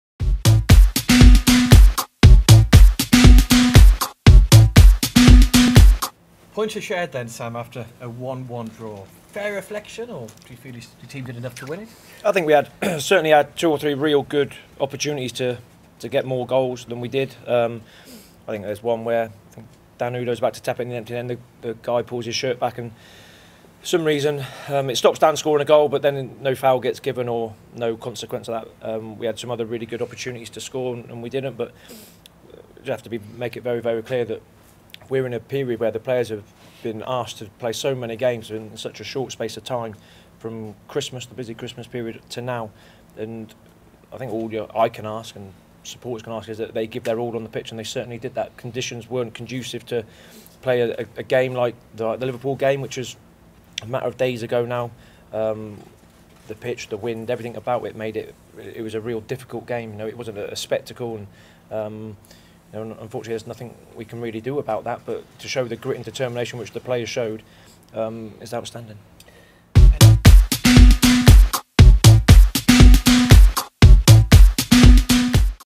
INTERVIEW | Sam Ricketts Post MK Dons - Town TV
Sam talks to the press after the draw at home to MK Dons - full interview available on IFollowShrews